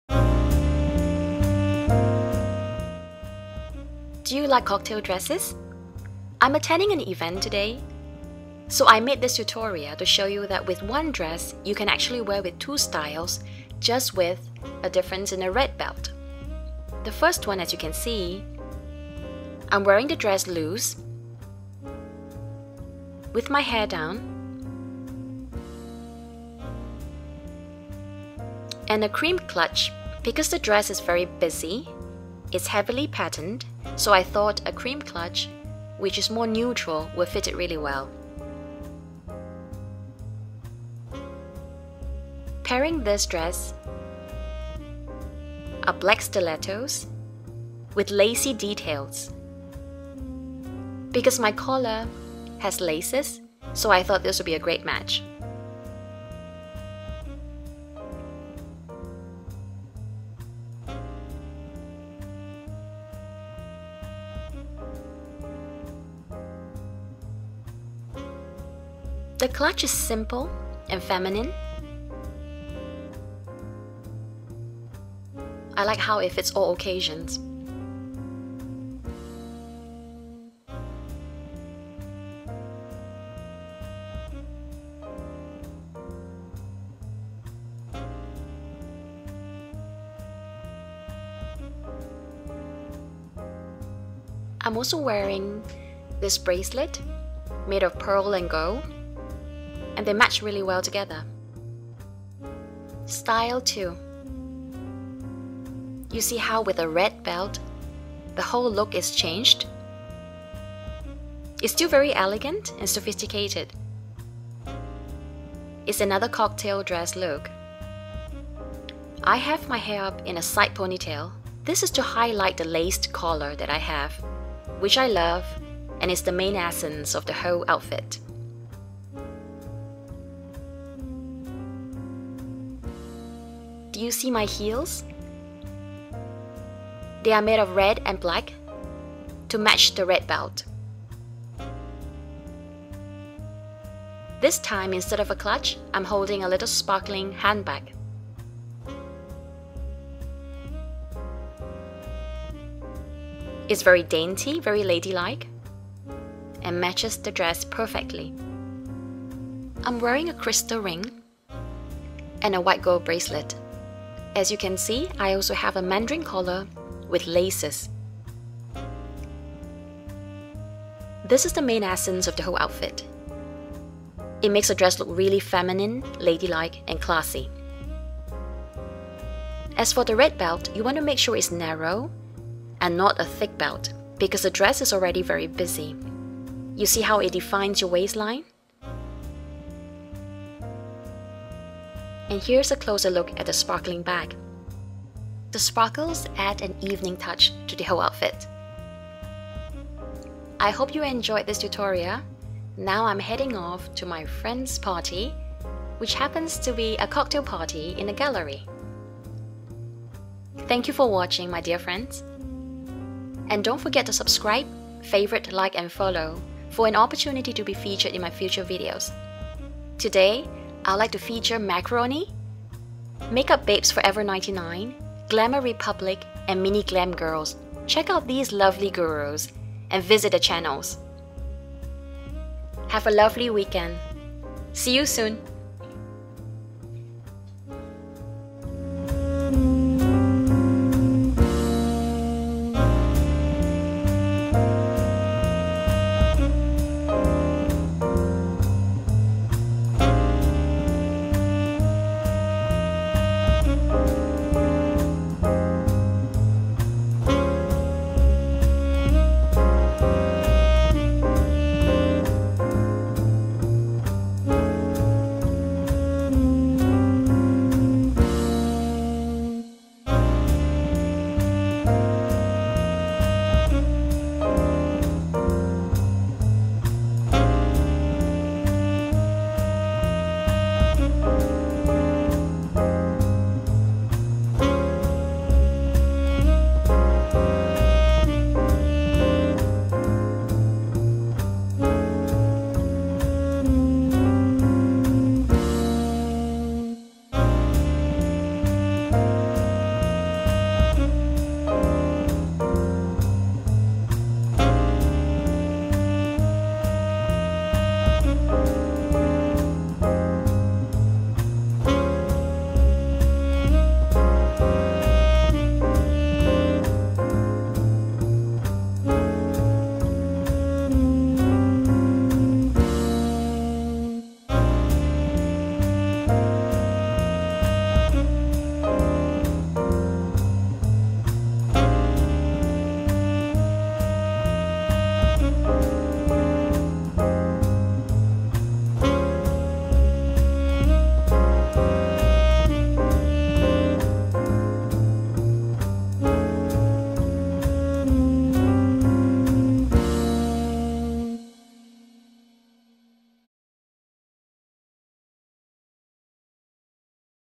ONE Beautiful Cocktail Dress: TWO Gorgeous Styles
*The channel links of the lovely gurus I am featuring are below*
I'm heading out for a cocktail party, I'm going to show you how I can wear my cocktail dress in 2 different styles- with the help of a red belt. Again, fashion is about mixing and matching-have fun with it ! Hope you will enjoy this video :)